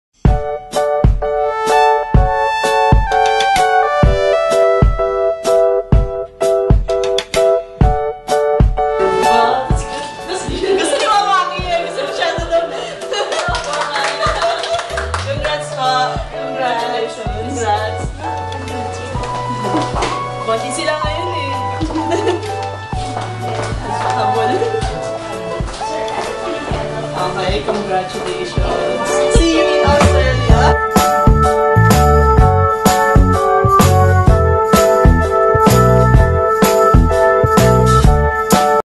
Let’s Spring In Delight With Richelle Kathleen Garcia
Read, Watch and Discover, as our Satisfied Filipino Applicants & Australian Sponsors share their stories about the remarkable migration service they experienced during their journey from the Philippines.